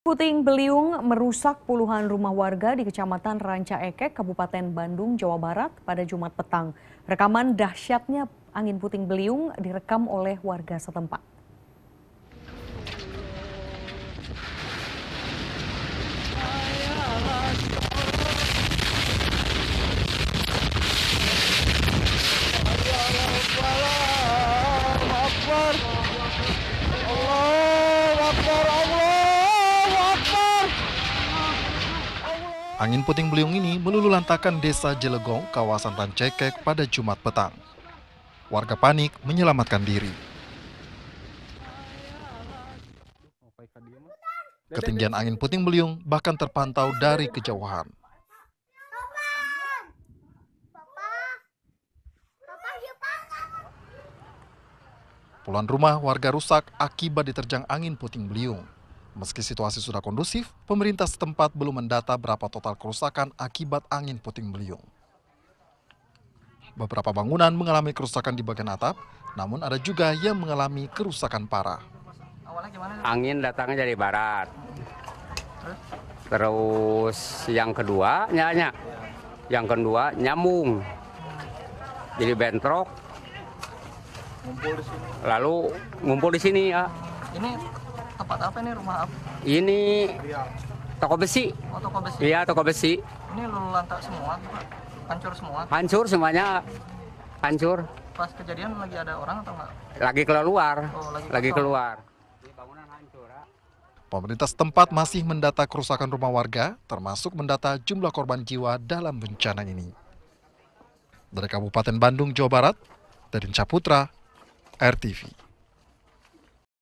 Detik - detik Puting Beliung Menyapu Rancaekek, Bandung
Angin puting beliung menerjang wilayah  Rancaekek, Bandung, Jawa Barat pada Jumat (11/1). AKibatnya ratusan rumah warga menglami kerusakan.